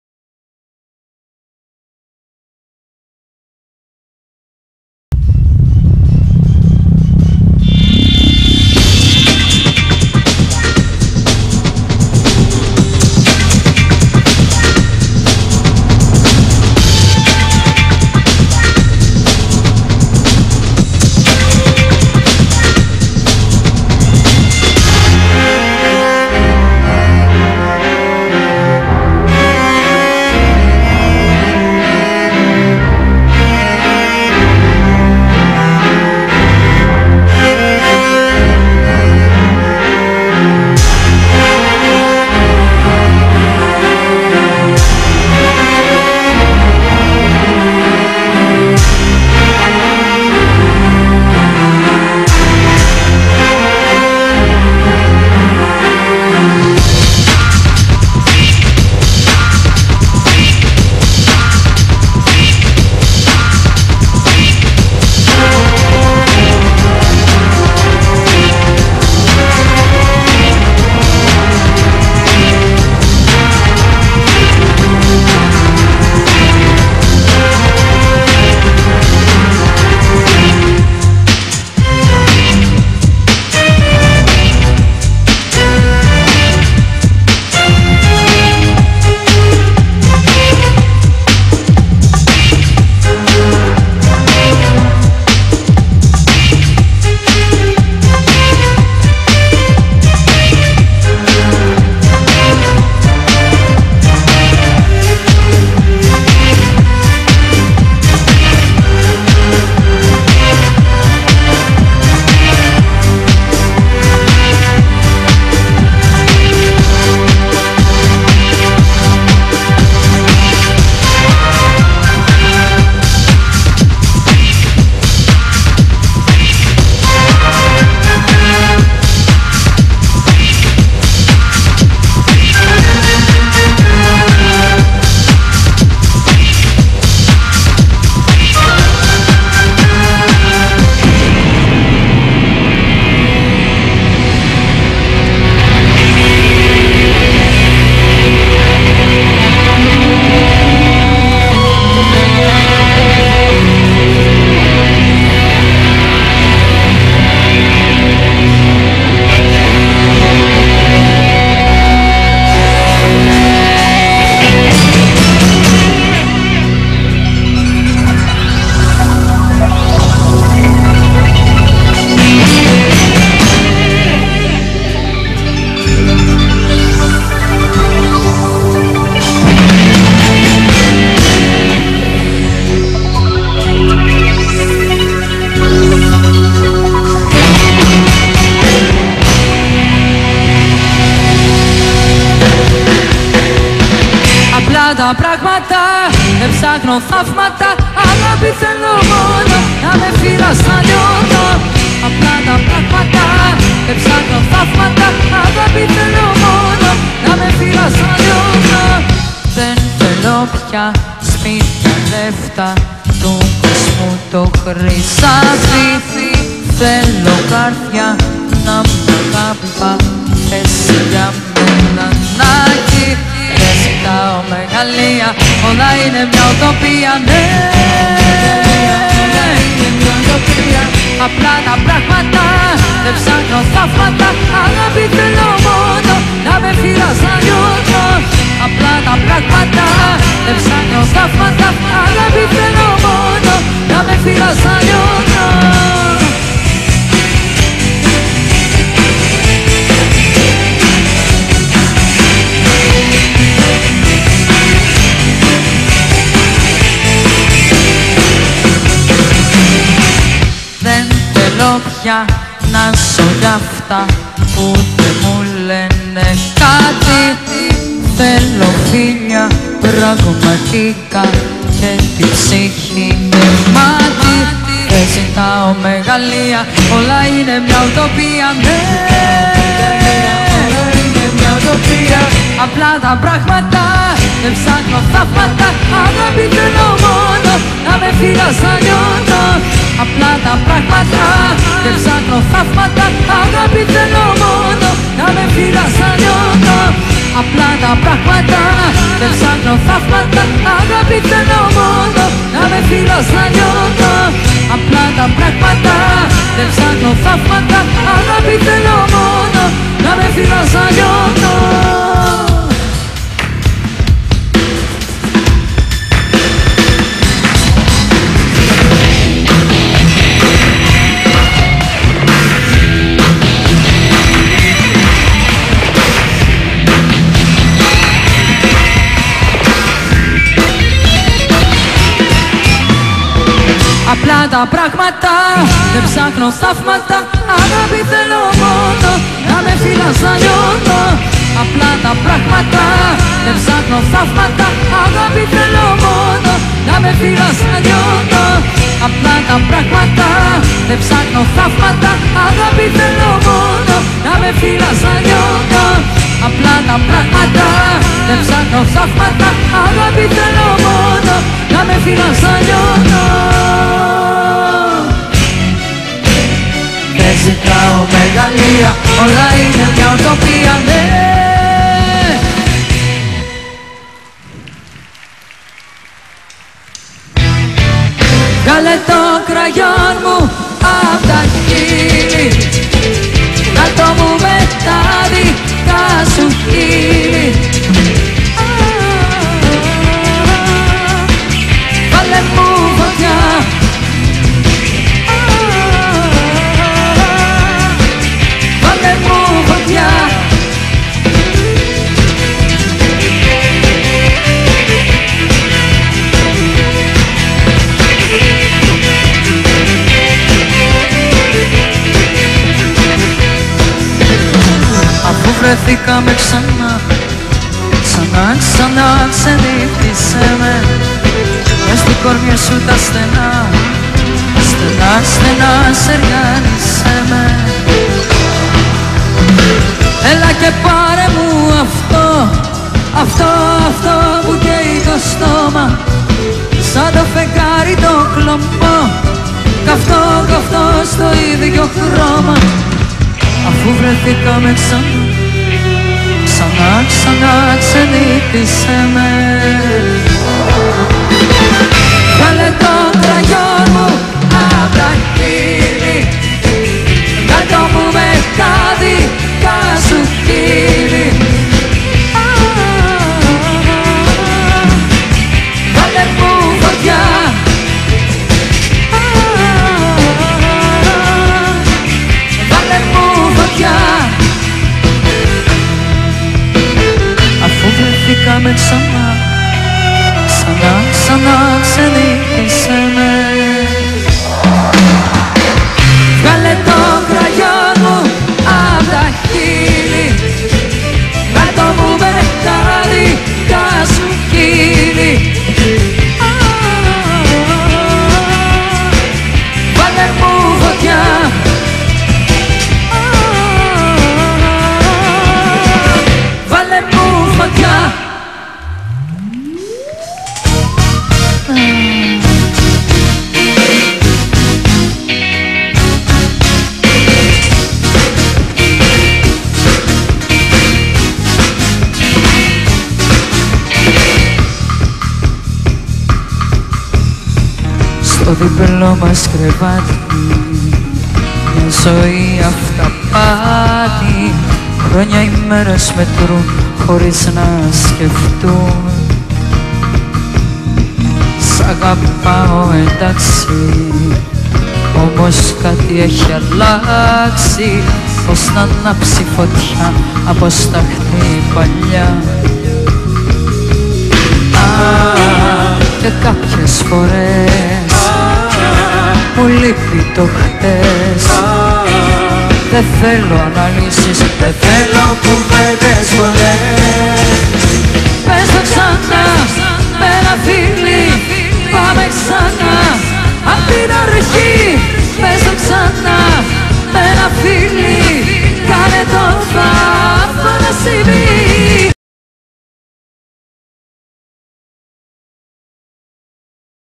ΚΑΙΤΗ - ΙΕΡΑ ΟΔΟΣ (ΜΕΡΟΣ Α)
Ένα από τα μεγαλύτερα σχήματα που πέρασε ποτέ από την Αθηναϊκή νύχτα ήταν αυτό της Καίτης Γαρμπή με τον Πασχάλη Τερζή στην Ιερά Οδό (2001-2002). Στο βίντεο μέρος του μοναδικού προγράμματος της Καίτης...